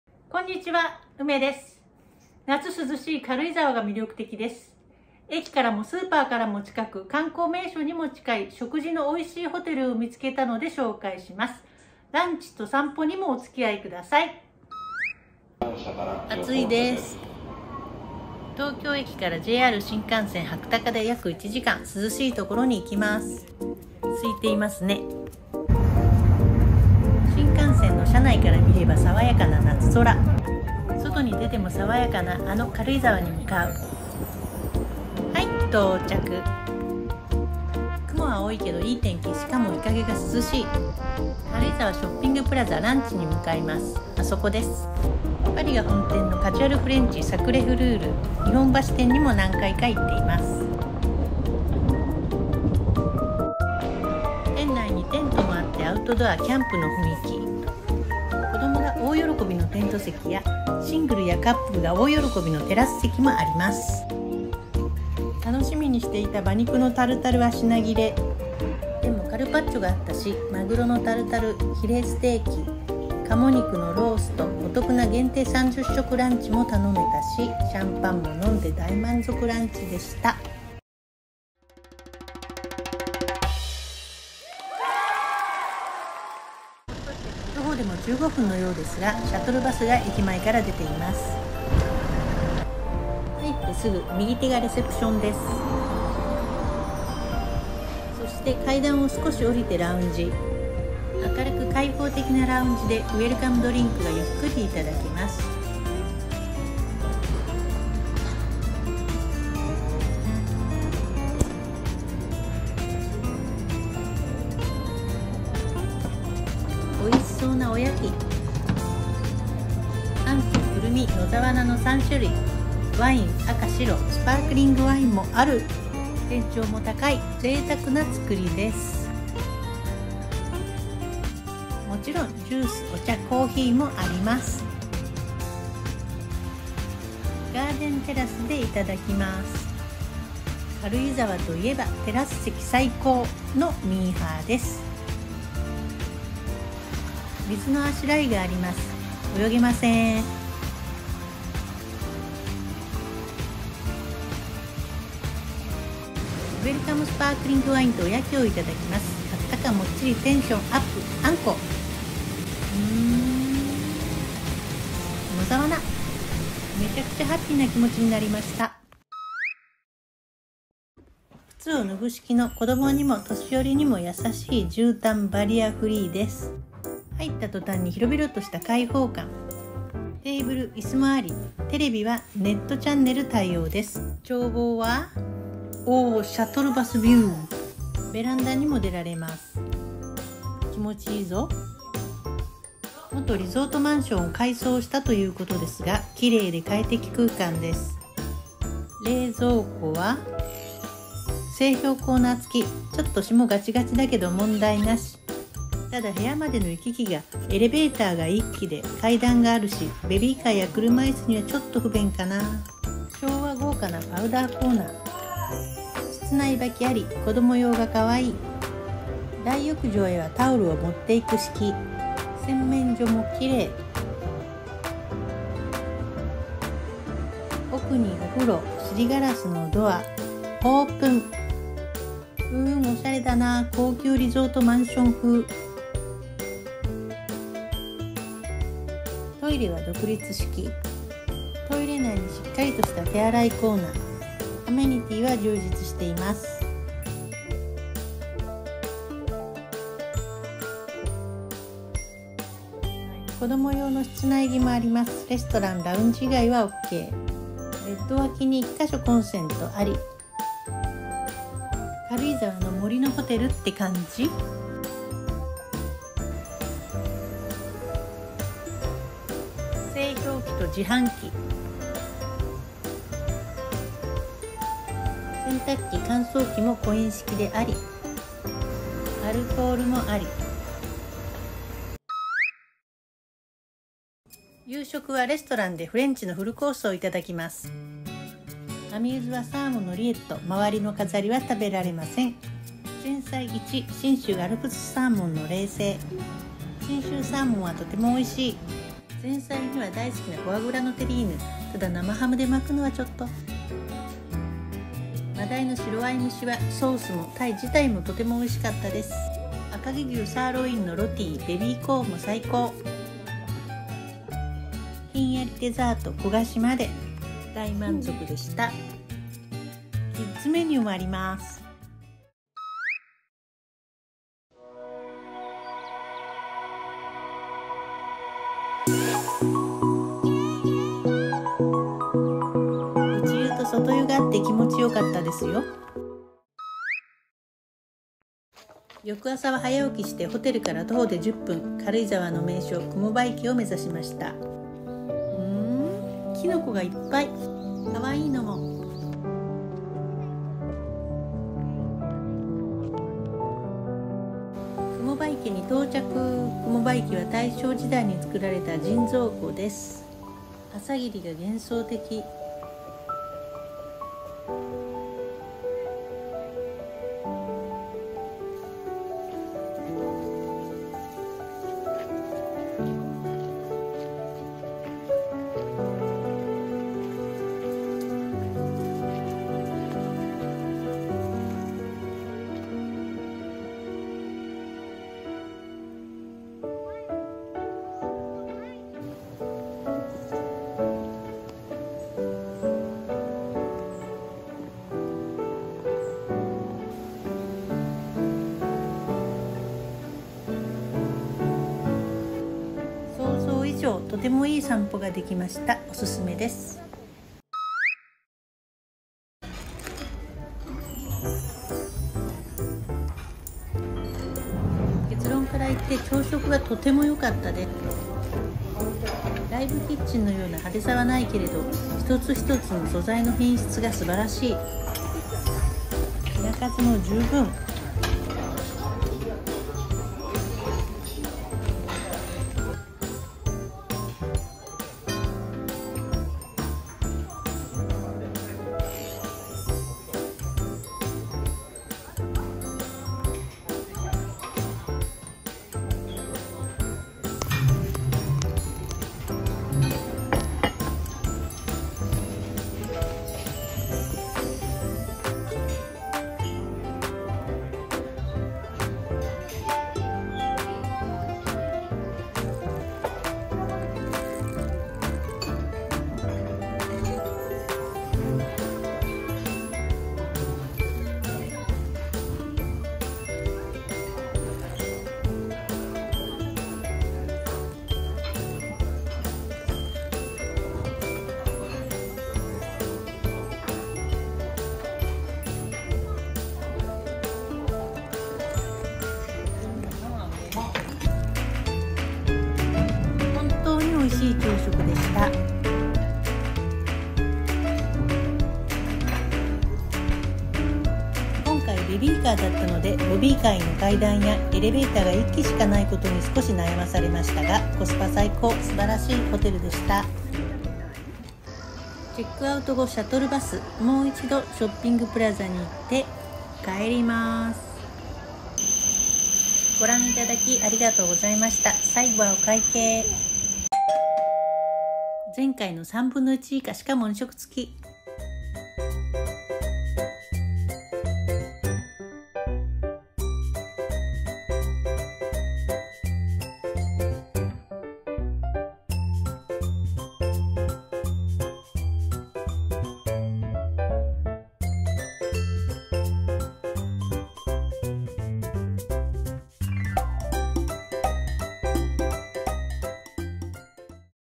【軽井沢ホテルサイプレス 雲場池】宿泊体験 立地が良くて食事が美味しい！コスパ◎
軽井沢で満足度の高いホテルです。
２食付きで前回の１／３以下の予算。
駅からも近い。スーパーも近い。
雲場池の散歩が想像以上でした。